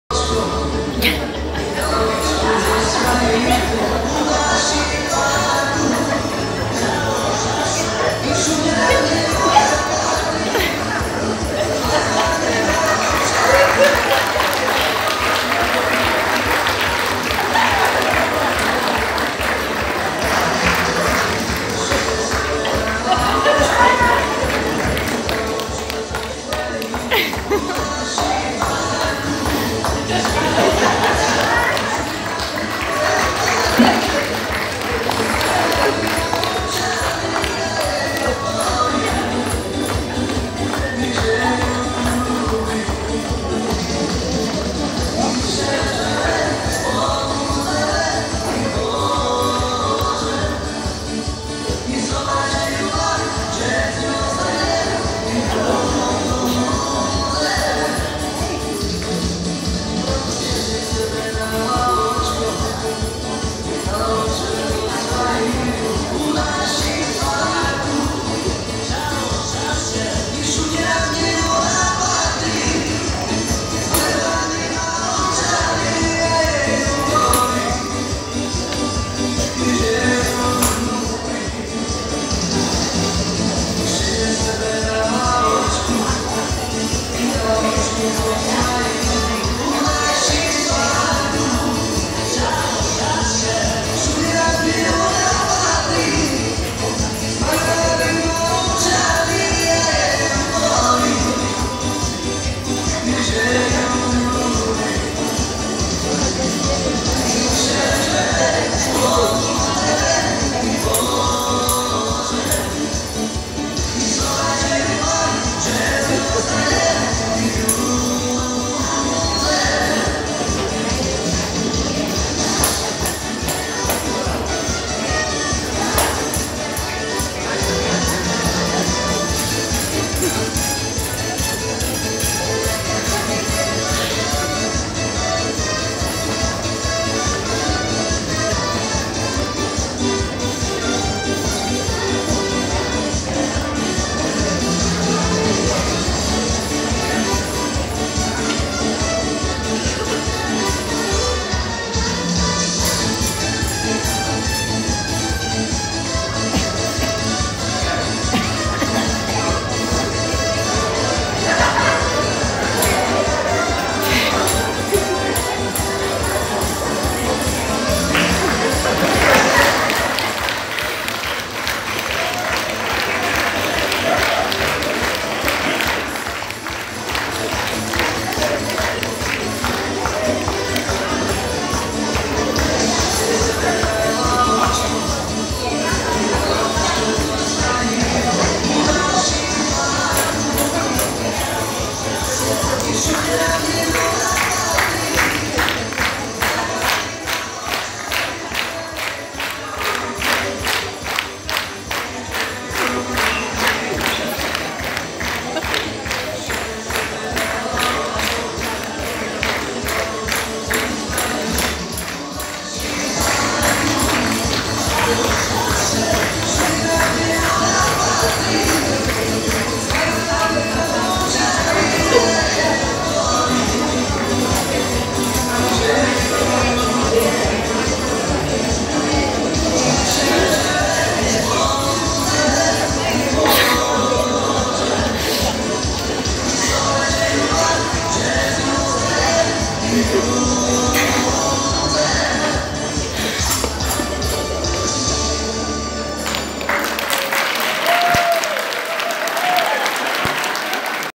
Materská škôlka Veľký Folkmar - Úcta k starším 2018 - vystúpenie
21. október 2018 - Dom kultúry Veľký Folkmar